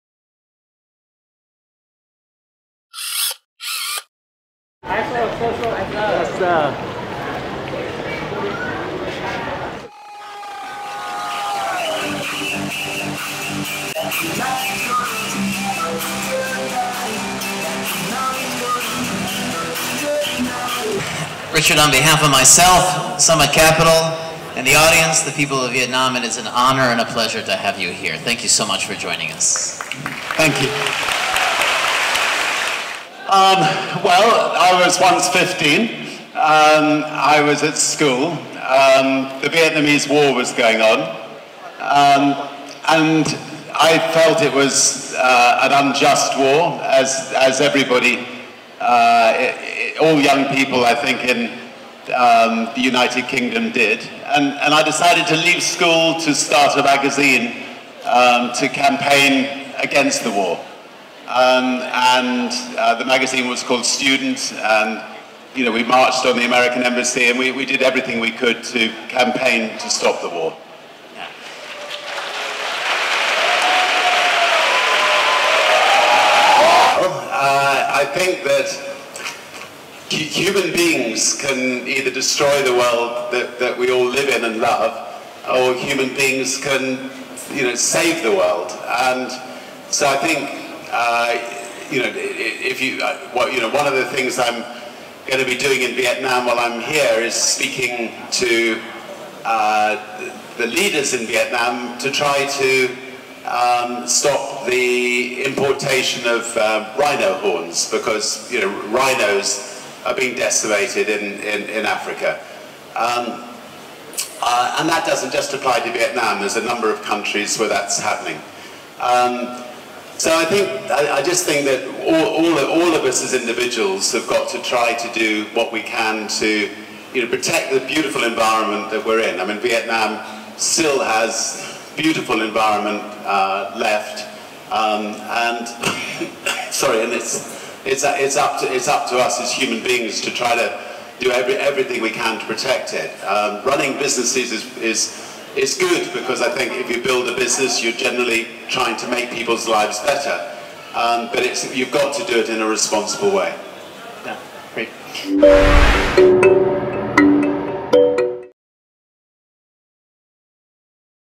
Sir Richard Branson inspires Vietnam's entrepreneurs
About 7000 budding entrepreneurs gave a fanatic welcome to Virgin founder Sir Richard Branson yesterday.

The tycoon appeared at the end of a two-day conference in Saigon filled with renowned speakers who shared their stories of accumulating enormous wealth.

The event heralded the arrival of the self-help business circuit to a country buzzing with start-ups and warmly embracing capitalism.

Sir Richard advised the country’s burgeoning business elite to remember their social responsibility among the many topics he covered.

Read next month’s AsiaLIFE magazine for a look some of the country’s most successful young entrepreneurs and look out for an all-encompassing video of MOVE Vietnam’s event later this week.